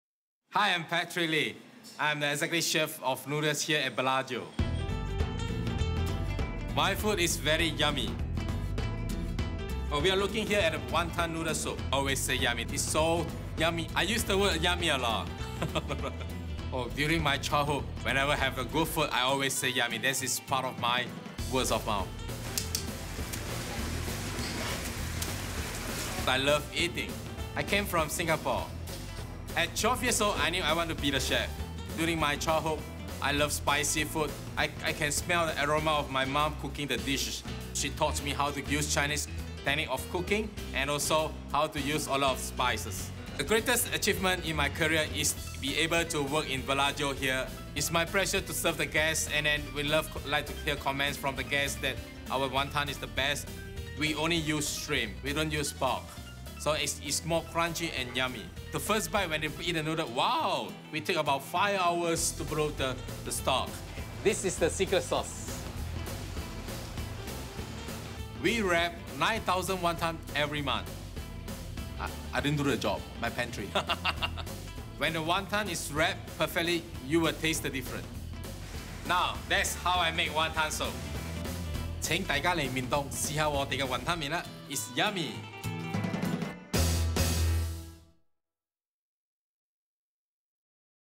Noodles
Chef Lee describes his inspiration for cooking...it's simply Yummy!